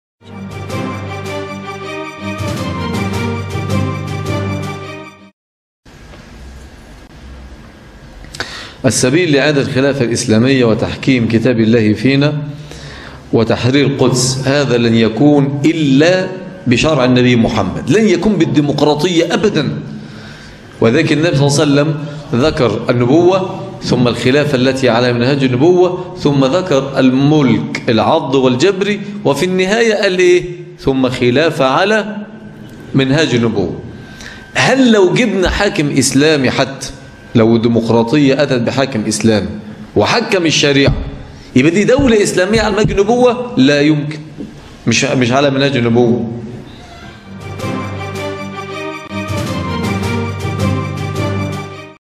Sheikh Ahmad Naqib: Khilafah volgens de methode van de Profeet (saw)
Sheikh Ahmad Naqib gaat in op een vraag over de weg richting de Khilafah naar het voorbeeld van de Profeet (saw).